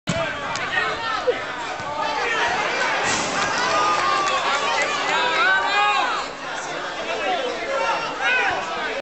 (Archive) Shot. Spanish Second Division. CD Raqui San Isidro - UD Villa Santa Brígida 18/5/2008
Shot. Archive images. Spanish football match. Imágenes de archivo. Second Division B League. Club Deportivo Raqui San Isidro - UD Villa Santa Brígida 18/5/2008. League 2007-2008. Grupo 1. Jornada 38. Campo de Fútbol Municipal de San Isidro. Granadilla de Abona. Tenerife. Final: 1-2.